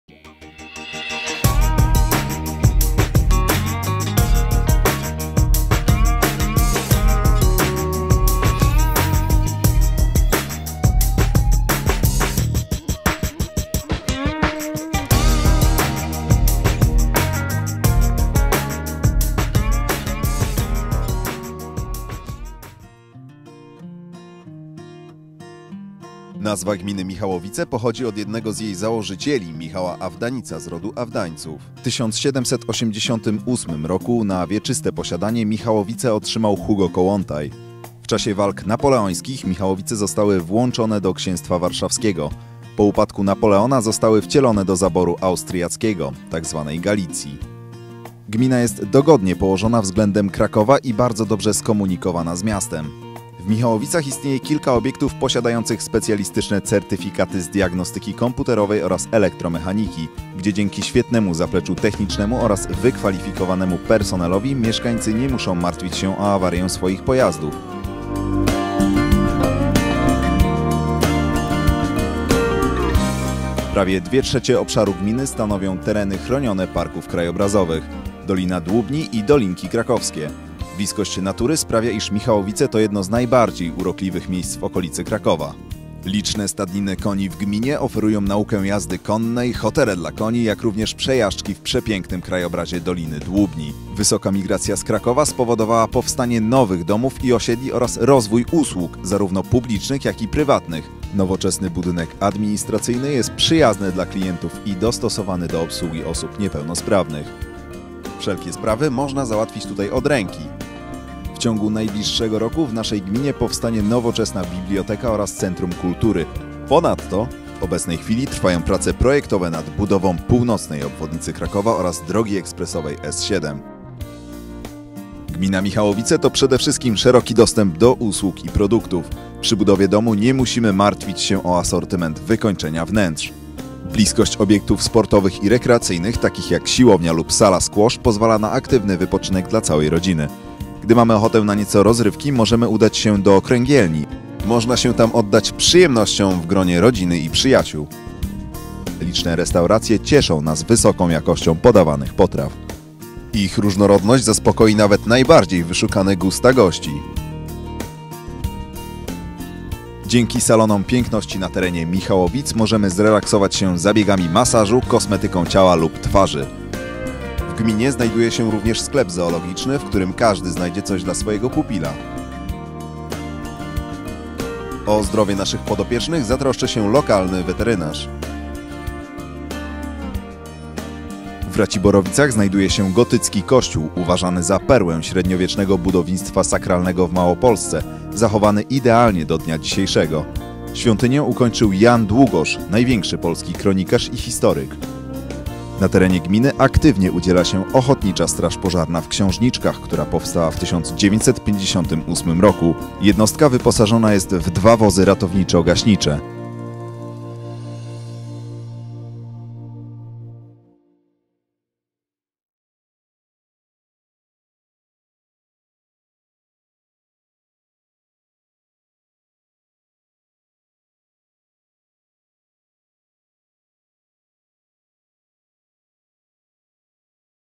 Michałowice Małopolska - film promocyjny
Film promujący gminę Michałowice.Gmina Michałowice zajmuje obszar o powierzchni 51 km², zamieszkały przez ponad 10 000 osób, skupionych w 19 sołectwach. Administracyjnie, Gmina Michałowice wchodzi w skład Powiatu Krakowskiego i Województwa Małopolskiego.
Od zachodu i południowego zachodu gmina graniczy z Gminą Zielonki, od północy z Gminą Iwanowice, od wschodu z Gminą Kocmyrzów-Luborzyca, od południa zaś na wąskim odcinku z krakowskim powiatem grodzkim, czyli z miastem wojewódzkim -- Kraków. Gmina Michałowice liczy około 10 000 osób.UWAGA !!! W filmowym tekście znajduje się błąd historyczny. Mianowicie gmina nie została wcielona do zaboru austriackiego ale do rosyjskiego za wyjątkiem Raciborowic (austriacki).